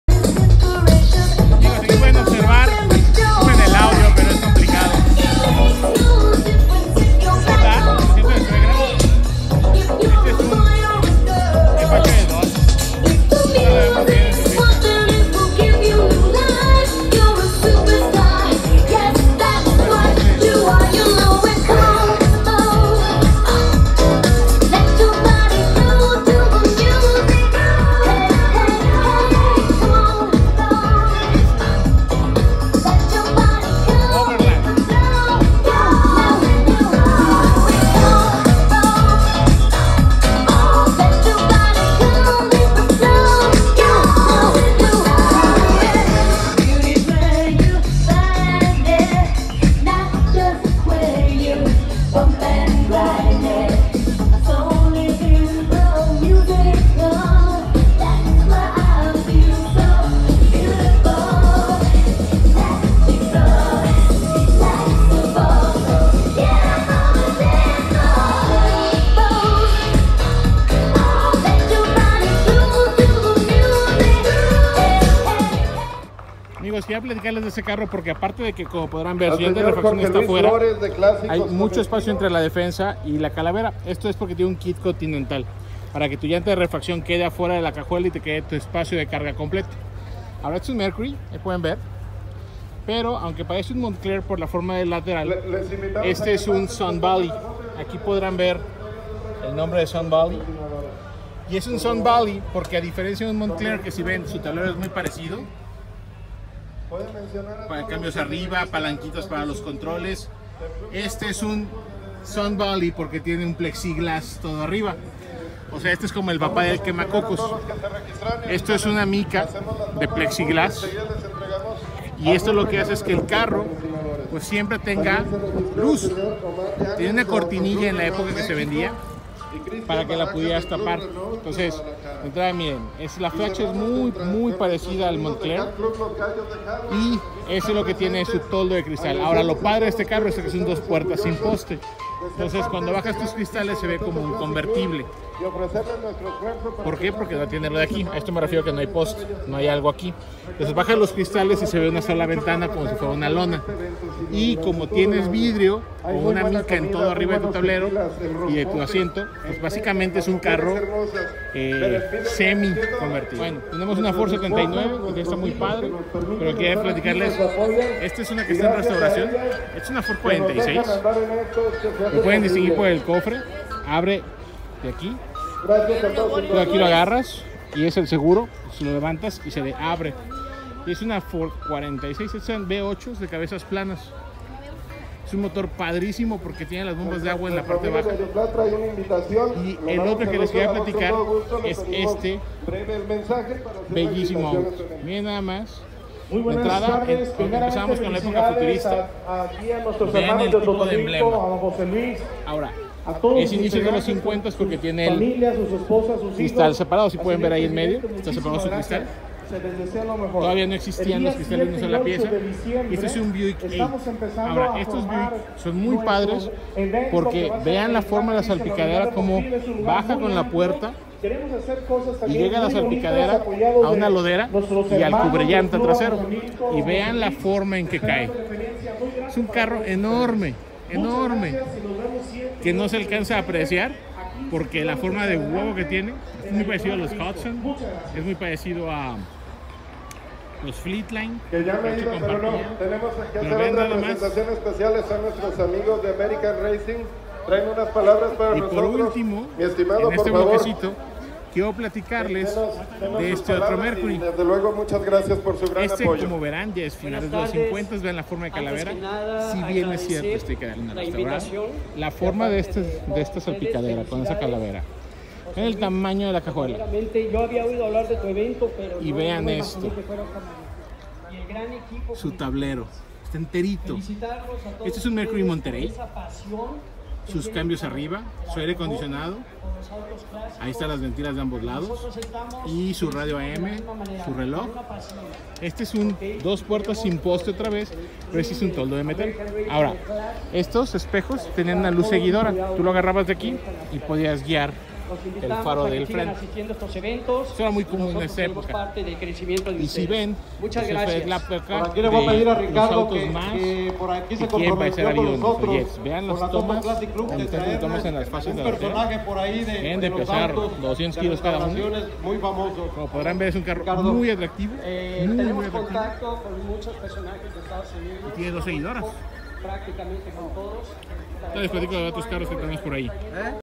Joyas de Atotonilco
Algunos autos nada comunes que vimos en el evento